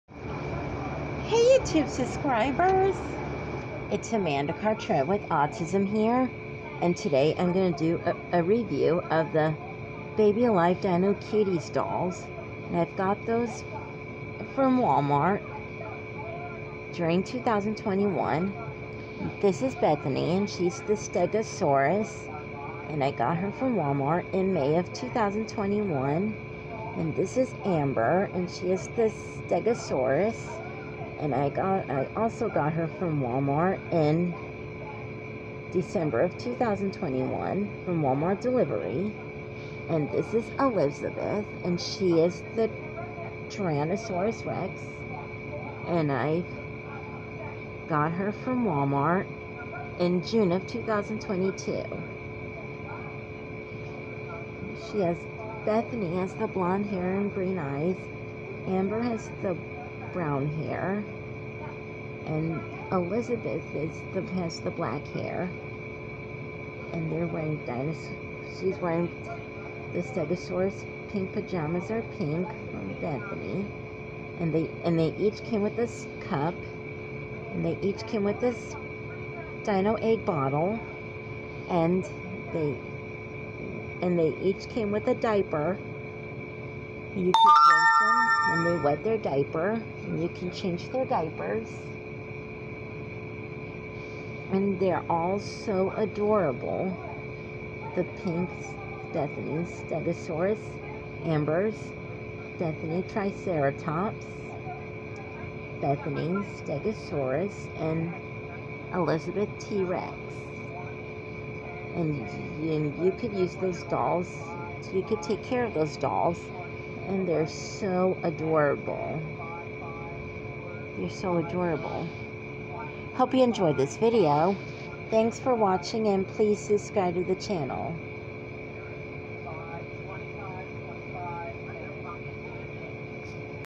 Baby Alive Dino Cuties Dolls Review
I Am Going To Do A Review Of The Baby Alive Dino Cuties Dolls.